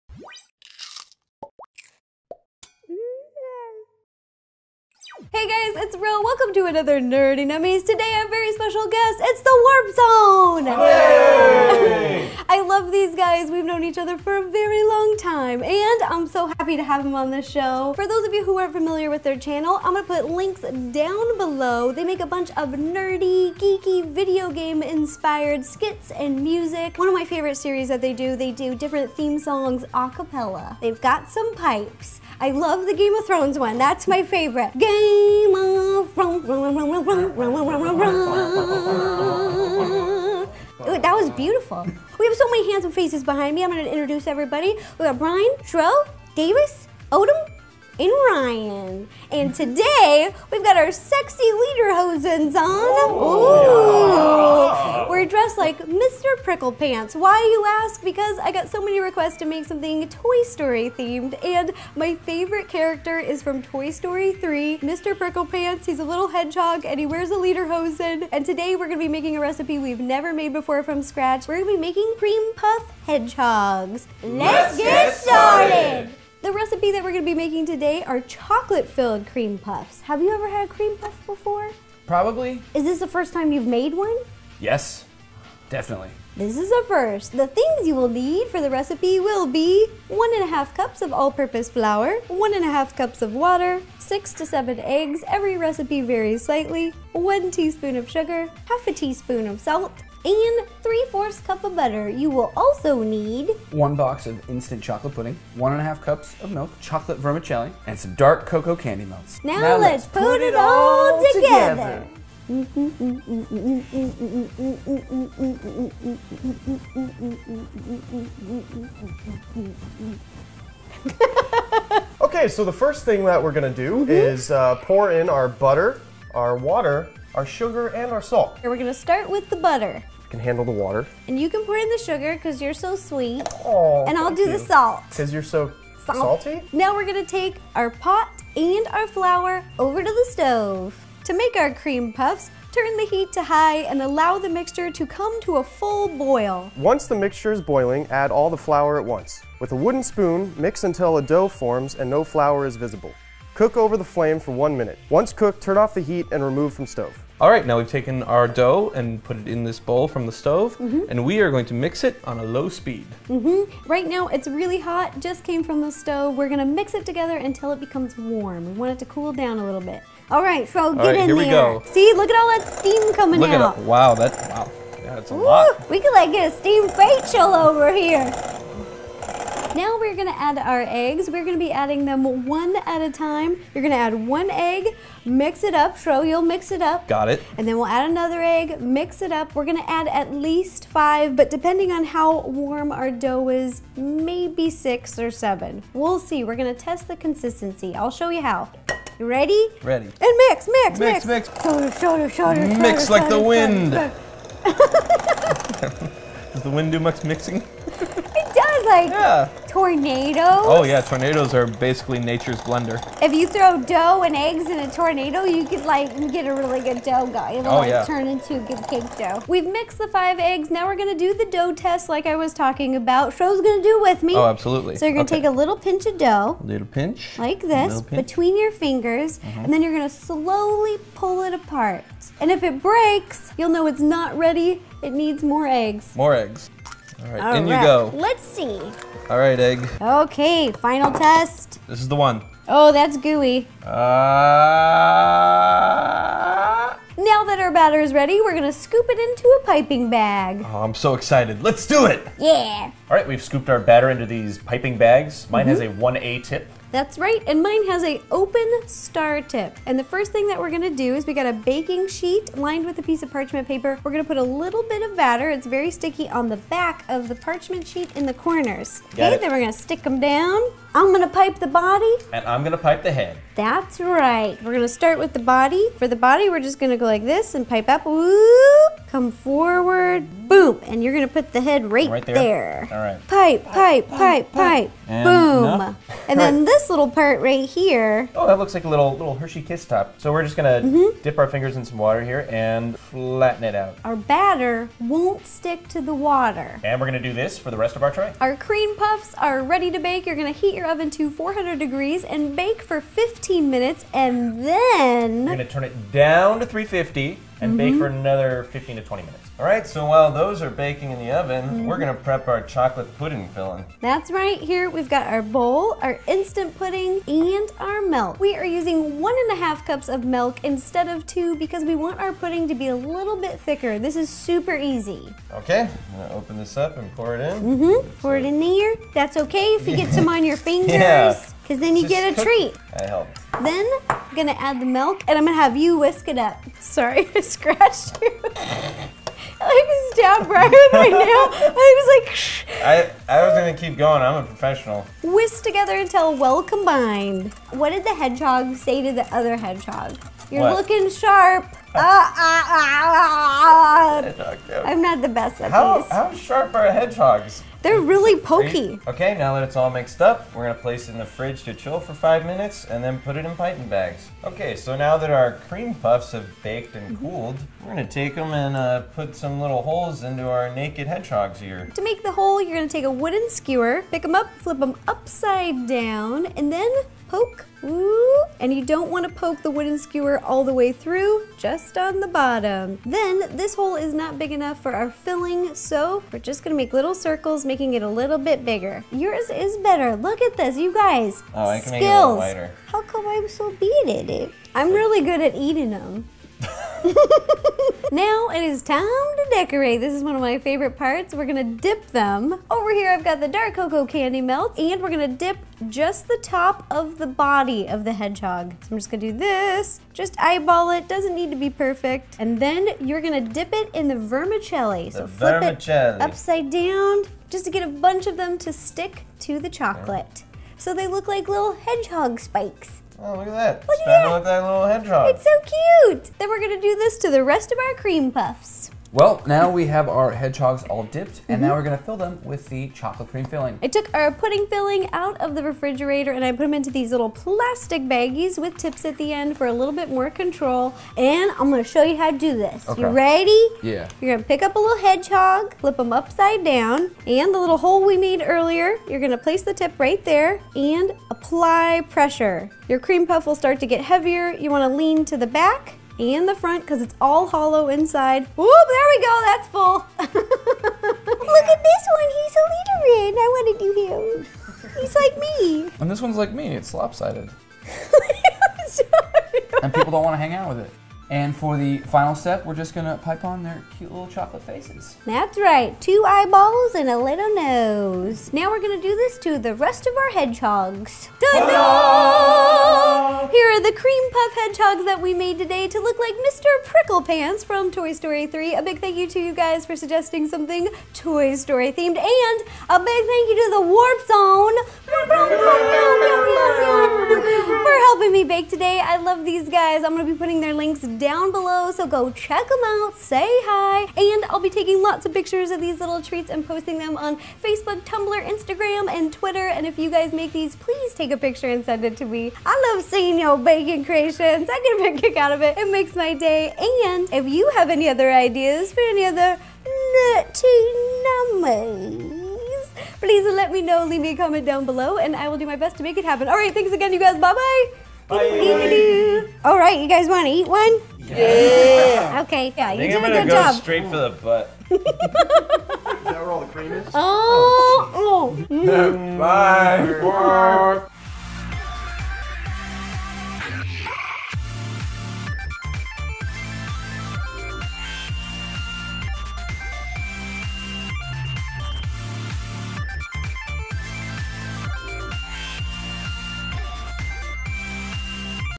HEDGEHOG CREAM PUFFS ft TheWarpZone! - NERDY NUMMIES
Today I made Toy Story 3 Mr. Pricklepants Chocolate Cream Puffs with help from The Warp Zone! Let me know down below what other videos you would like to see!

This is not a sponsored video. All products bought by me.

THE THINGS YOU WILL NEED:
* (1-1/2) Cups All Purpose Flour
* (1-1/2) Cups Water
* (6-7) Eggs
* (1) Teaspoon Sugar
* (1/2) Teaspoon Salt
* (3/4) Cup Butter
* (1) Box Instant Jell-O Pudding
* (1-1/2) Cups Milk
* (1) Container Chocolate Vermicelli
* (1) Bag Dark Cocoa Candy Melts